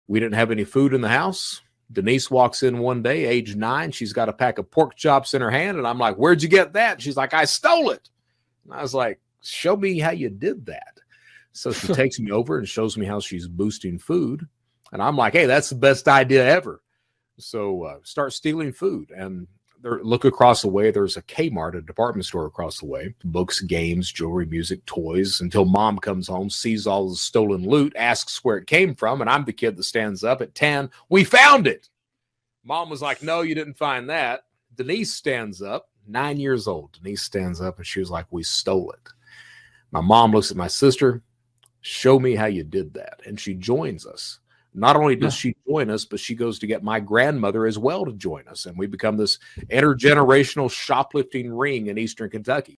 Grandma joins family crime gang!
Grandma joins Brett Johnson's crime gang after he learns from his sister how to steal.